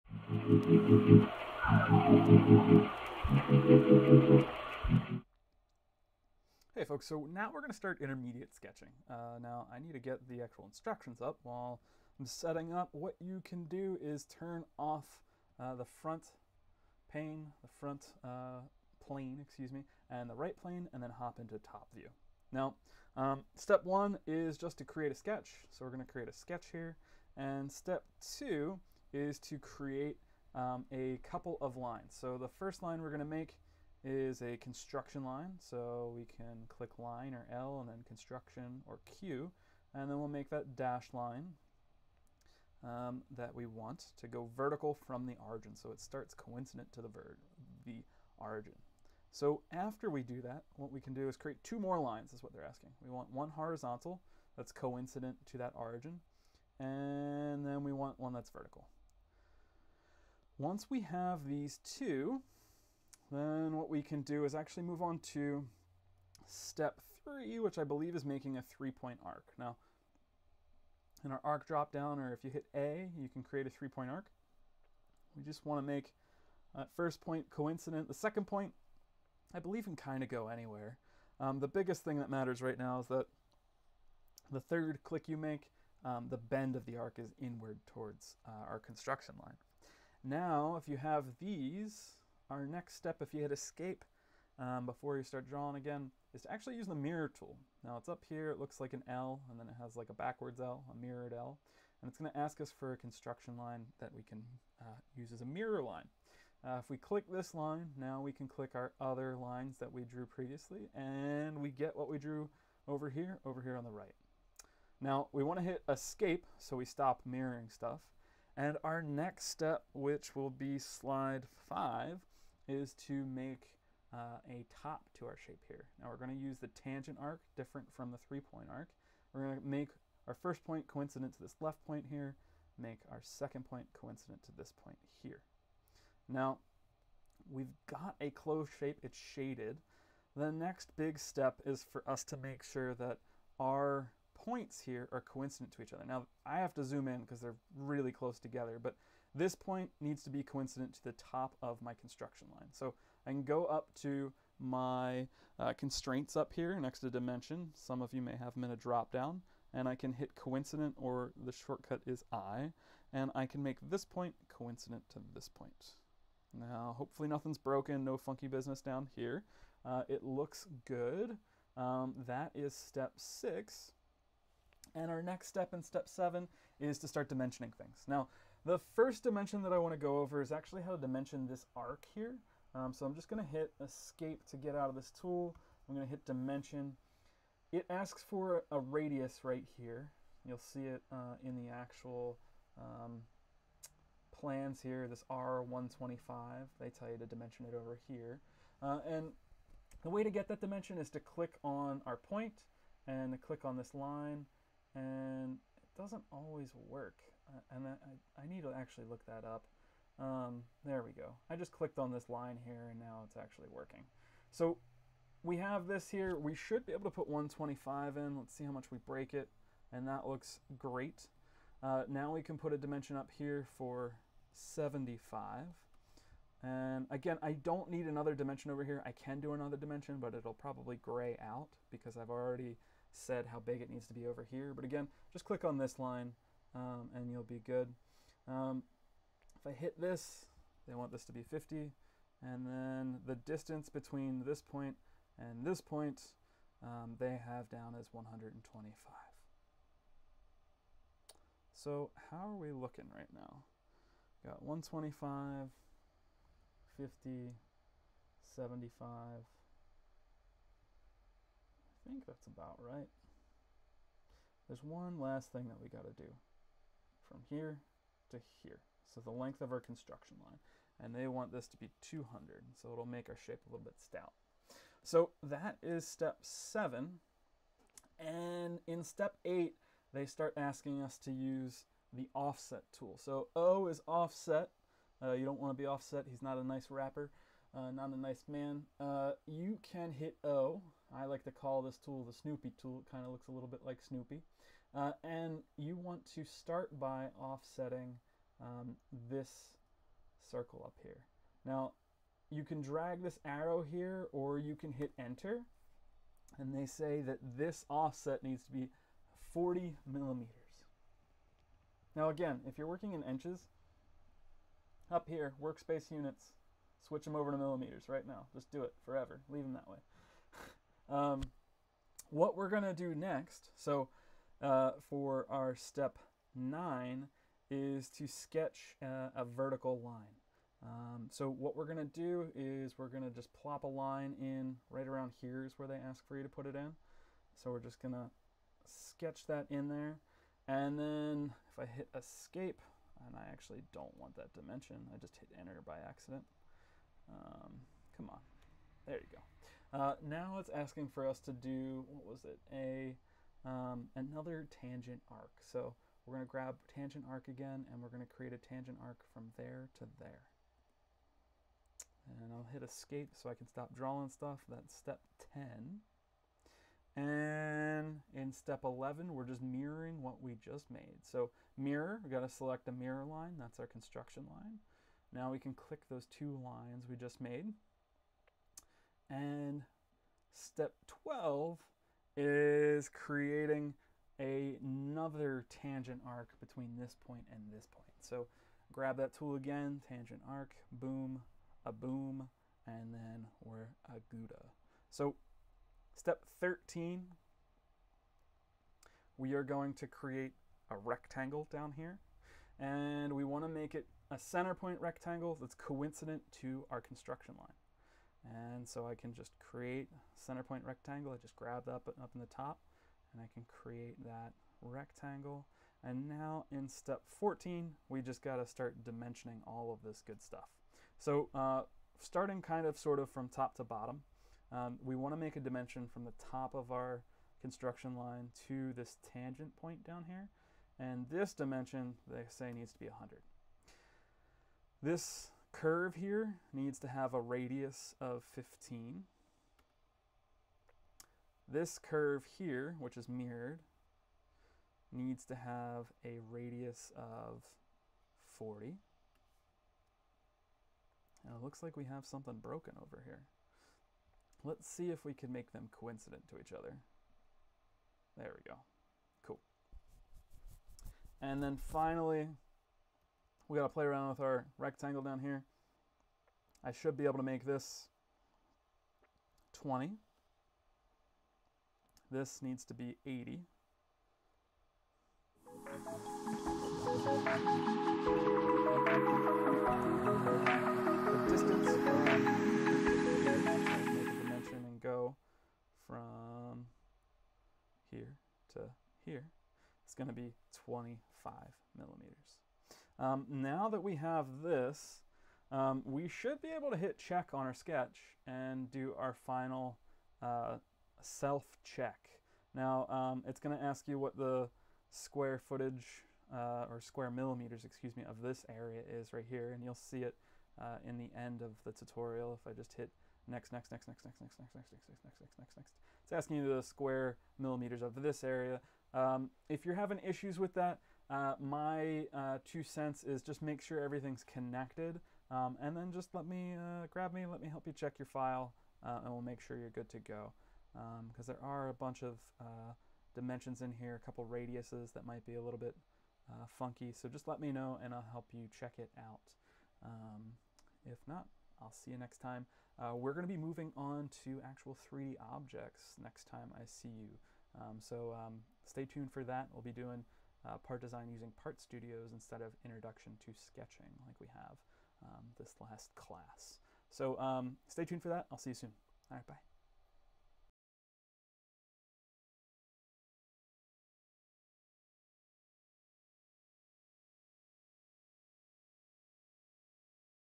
Exercise 2: Intermediate Sketching
Exercise 2 of the Introduction to Sketching module of the "Onshape Fundamentals: CAD" pathway on learn.onshape.com